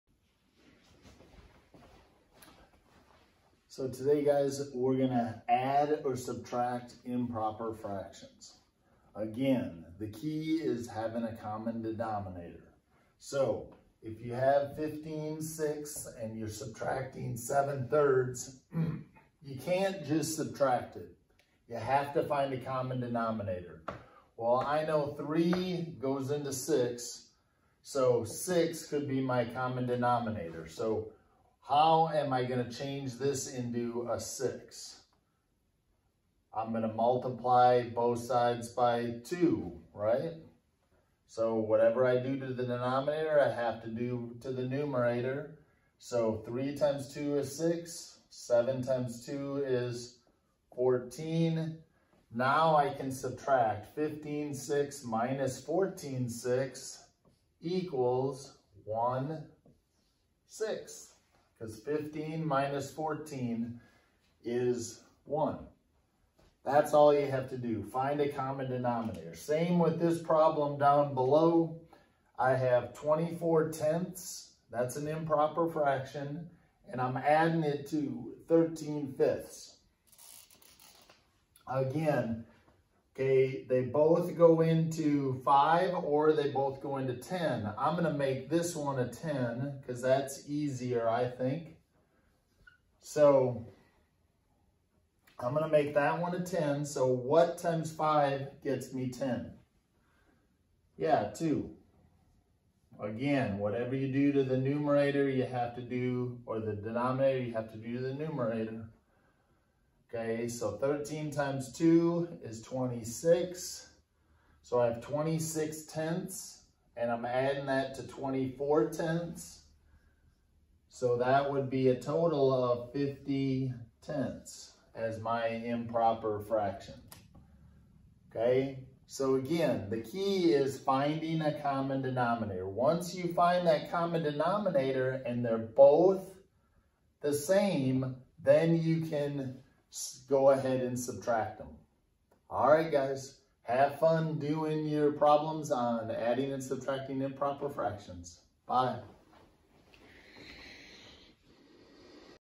Adding and Subtracting improper fractions
Students will learn how to add and subtract improper fractions.